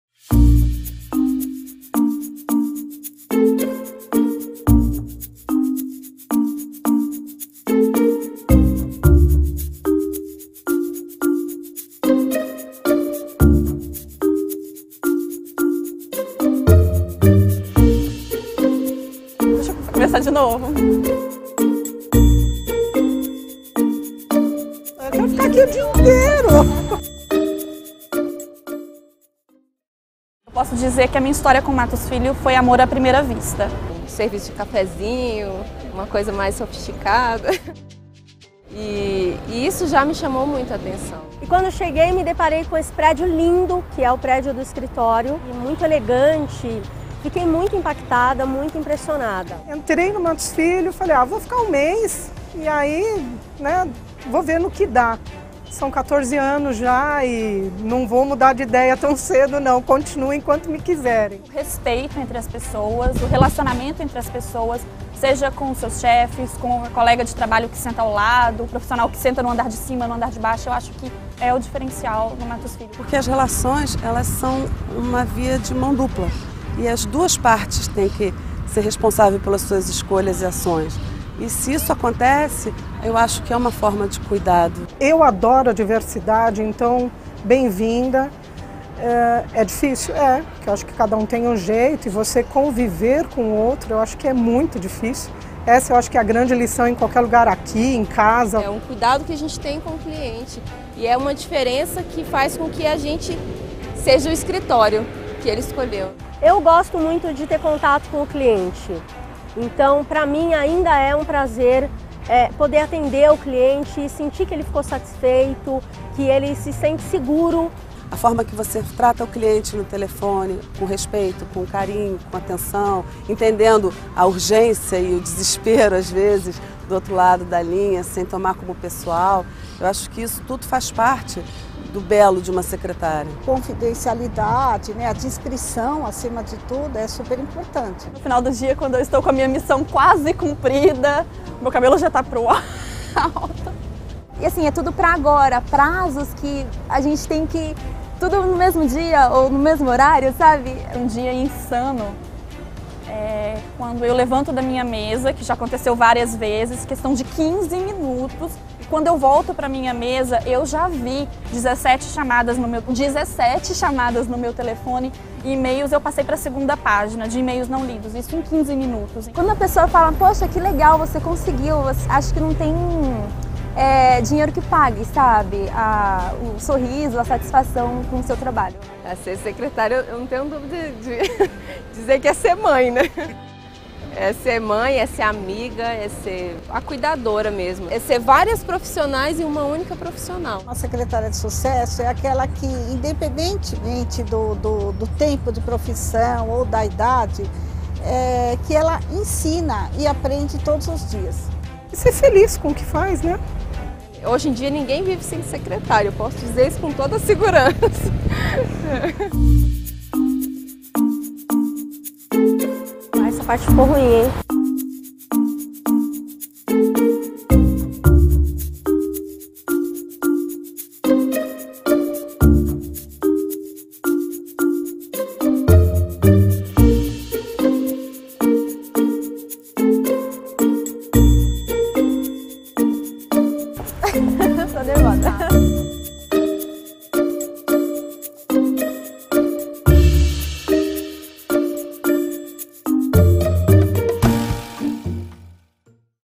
Dia da Secretária - Marcia Tortora (set/2014)
Vídeo em homenagem ao Dia da Secretária do Mattos Filho Advogados.
Praia do Flamengo - 30 de setembro de 2014